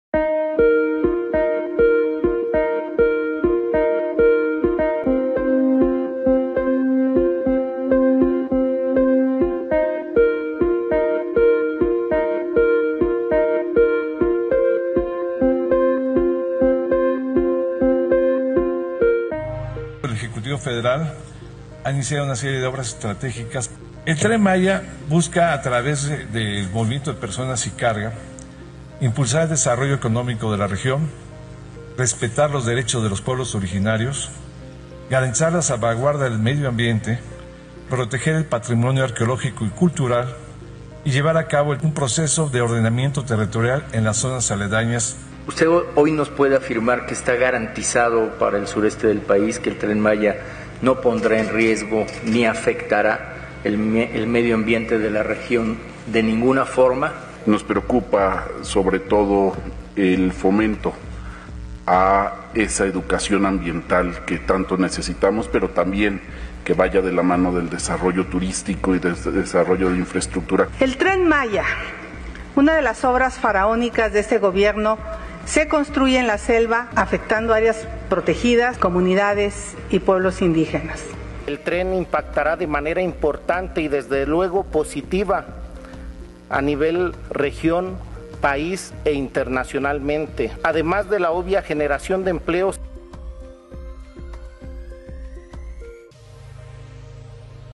Comparecencia del Director General de FONATUR, Milardy Douglas Rogelio Jiménez Pons, Glosa 3er...
Comparecencia del Director General de FONATUR, Milardy Douglas Rogelio Jiménez Pons, Glosa 3er Informe de Gobierno / Senado de la República